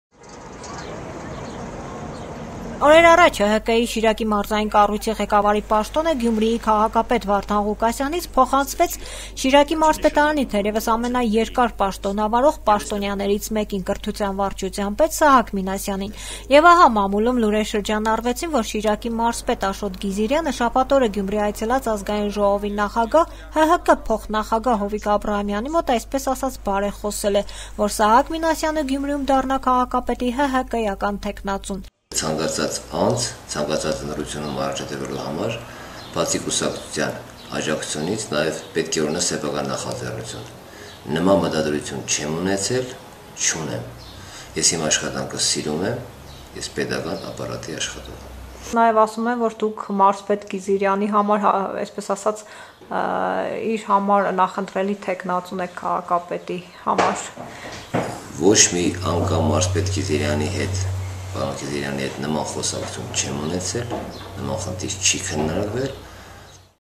Սահակ Մինասյանը հերքում է իր թեկնածու լինելու լուրերը
ՀՀԿ Շիրակի մարզային կառույցի նորանշանակ ղեկավար Սահակ Մինասյանը հերքում է Գյումրիի քաղաքապետի թեկնածու դառնալու իր հավակնությունների մասին լուրերը: